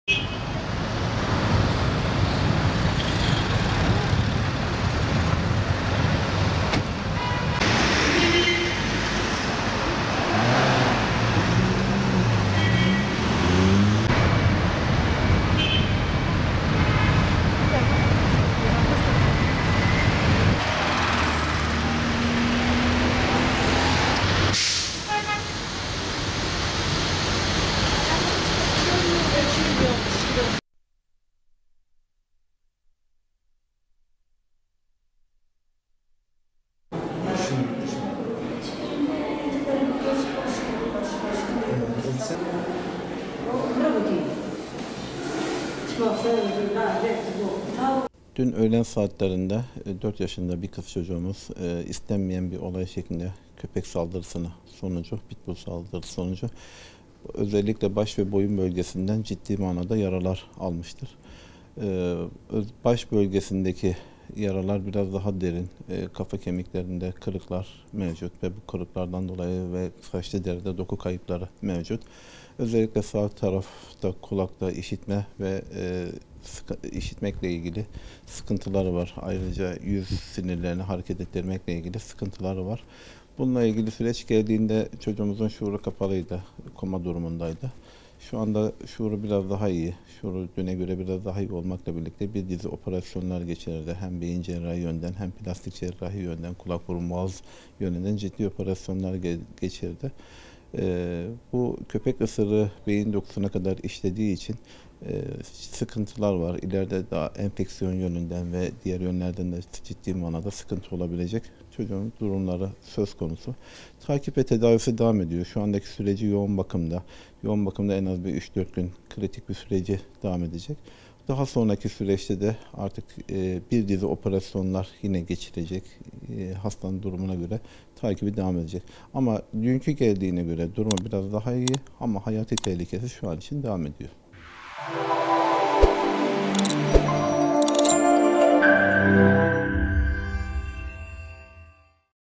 Pitbull cinsi köpeklerin saldırısına uğrayan çocuğun hayati tehlikesi sürüyor
Gaziantep'te bir sitenin bahçesinde pitbull cinsi iki köpeğin saldırısı sonucu ağır yaralanan 4 yaşındaki Asiye Ateş'in hastanedeki tedavisi devam ediyor.

Değerli İzleyicilerimiz;